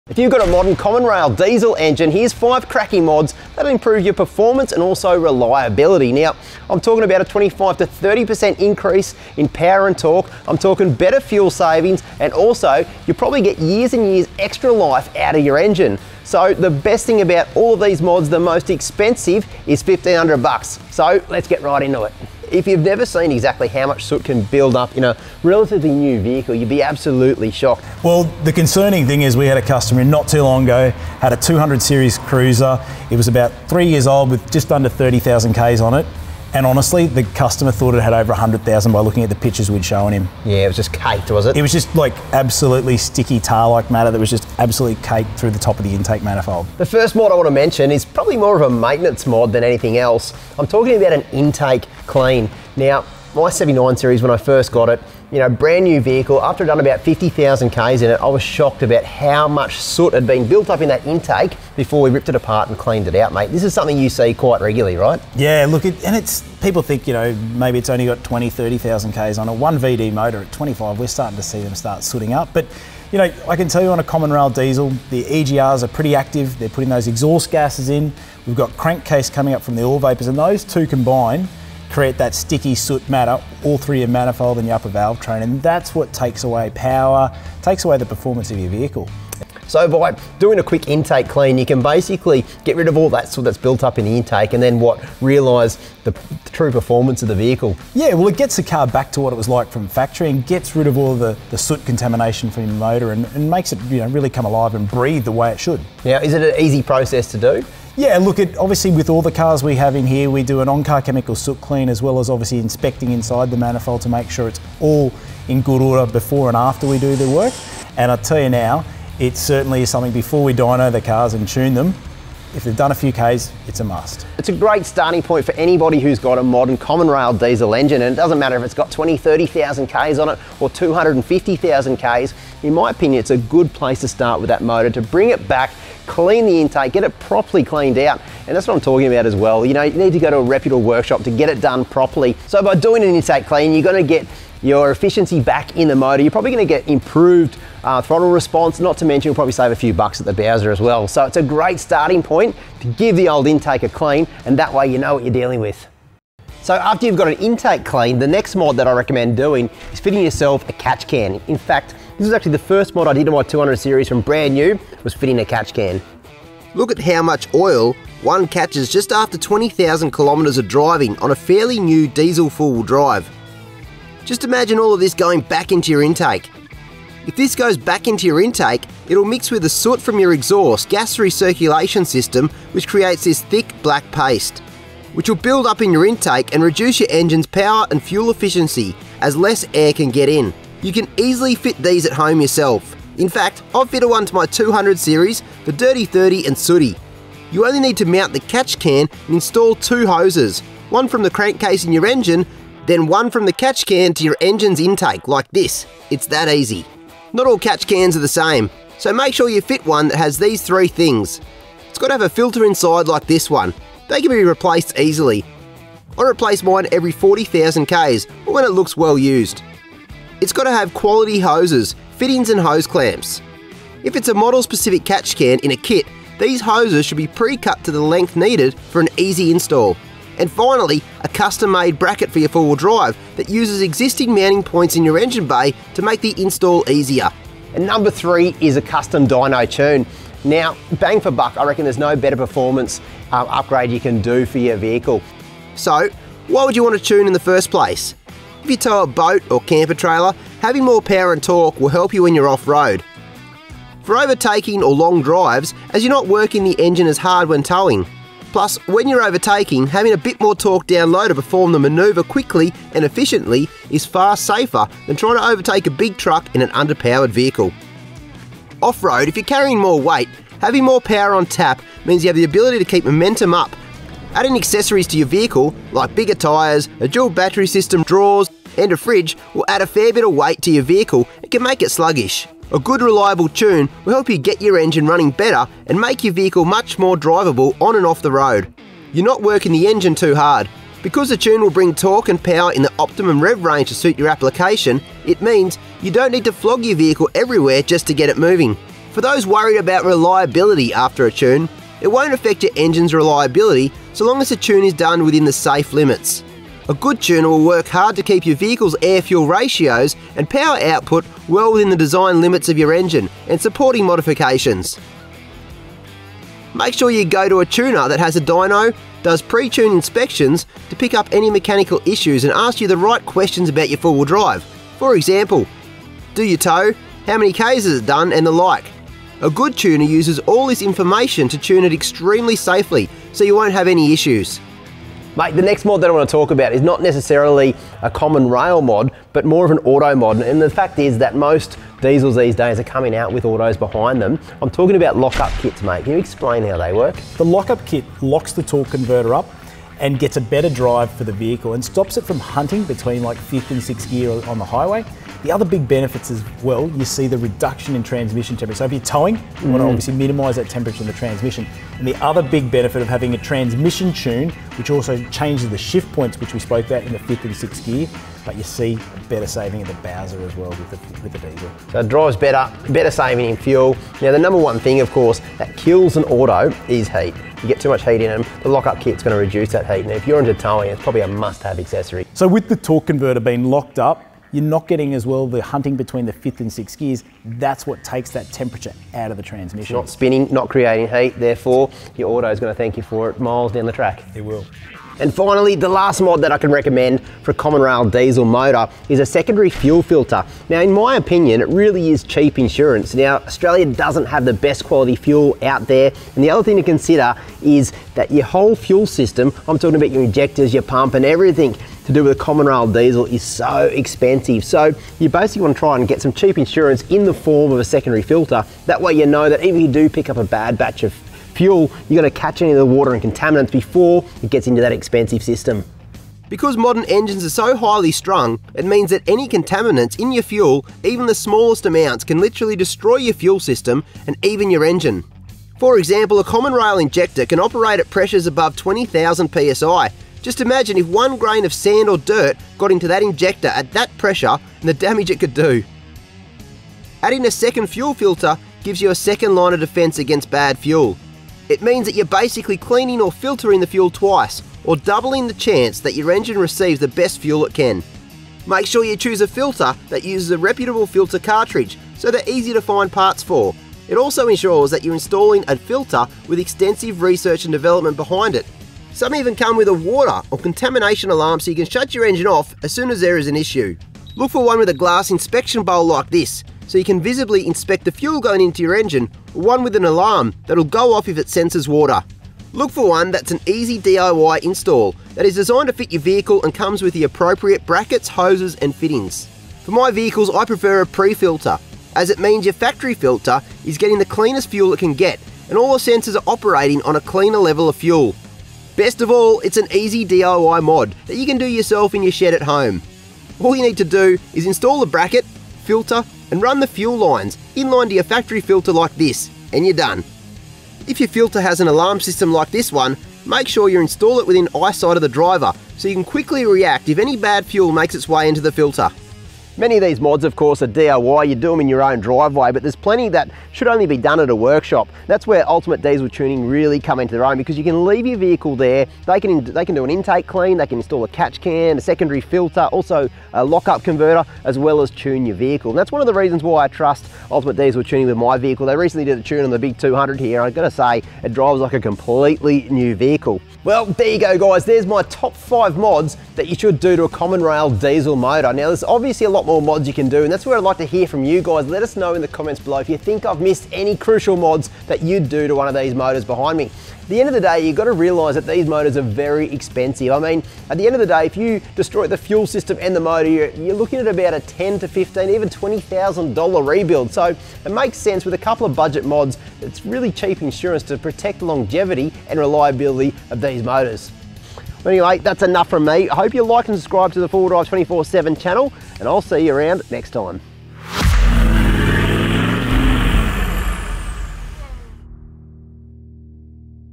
5 MODS EVERY MODERN DIESEL OWNER SHOULD DO – all under $1,500! Get 30% more power, more reliability
Shauno's got THE list of mods you can make to your diesel 4WD to get safe & reliable POWER! Dodgy tunes and cheap parts can cause thousands of dollars in damage, and even write off your 4WD.

To talk to an expert about making more power with your 4WD, talk

Car park or cowboy tunes are never a good idea. ONLY get your diesel 4WD tuned by dyno tuning experts, and only use top quality performance parts if you're really going for it!